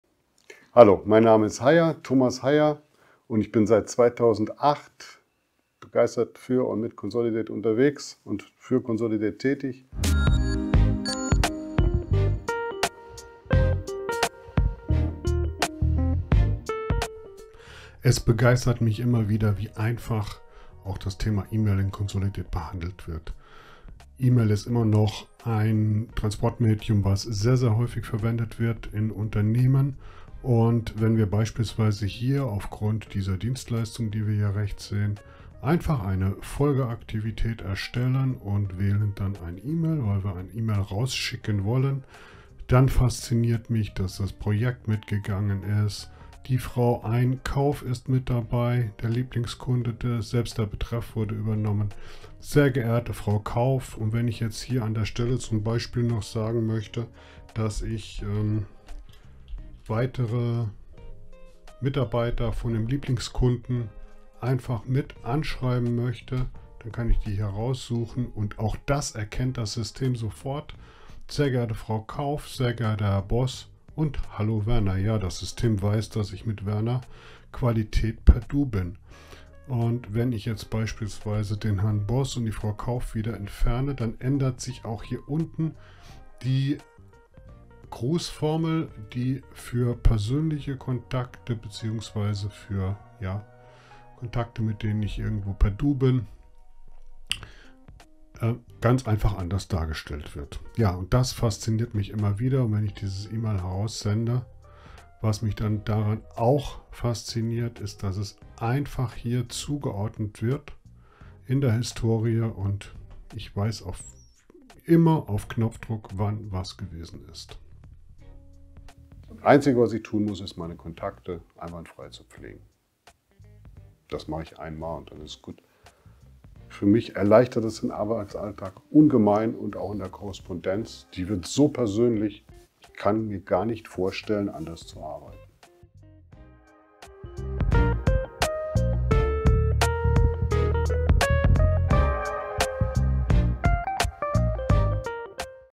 ► Persönliche E-Mails schreiben - So sehr begeistern mich E-Mails in Consolidate
Um auch Ihnen den Arbeitsalltag zu erleichtern, zeigt Thomas heute, wie man schnell und effektiv E-Mails in Consolidate schreibt.

Noch mehr Informationen findest du auch in unserem Handbuch